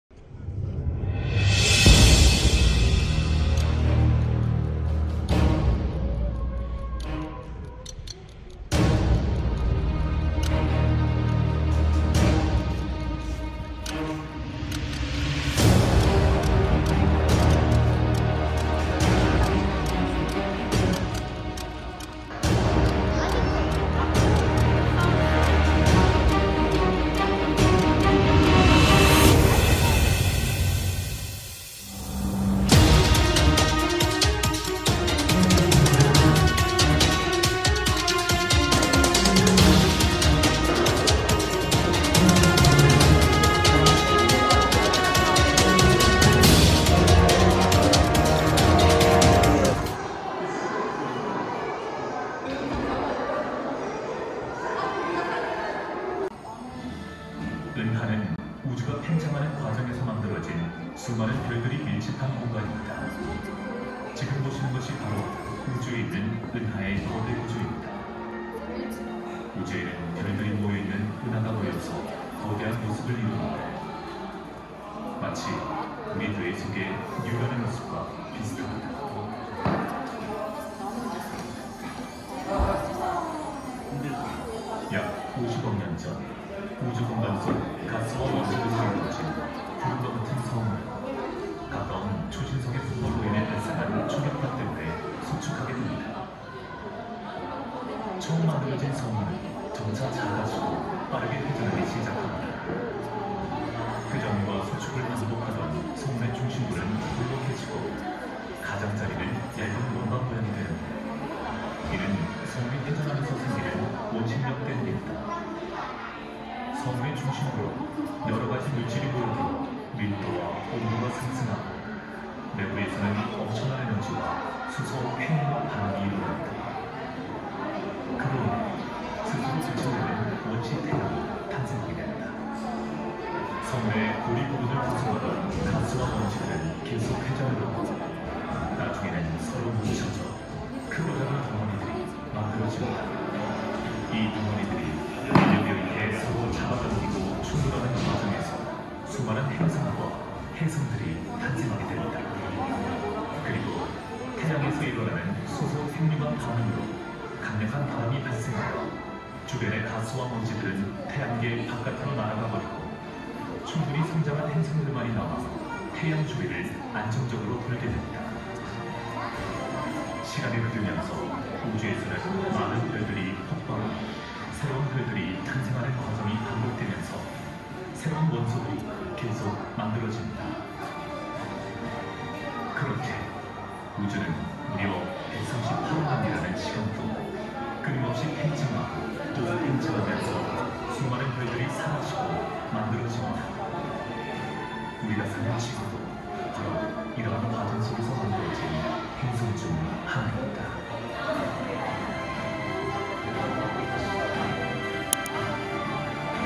Seodaemun Natural History Museum 서대문 자연사박물관 서울 한국 (Seoul, Korea)
Seodaemun Museum of Natural History, located in the heart of Seoul, is a place of learning for students, a place of culture for residents, and a place of leisure for families. They aim to provide an opportunity for city dwellers to experience animals and plants, and to help people learn to appreciate and love nature.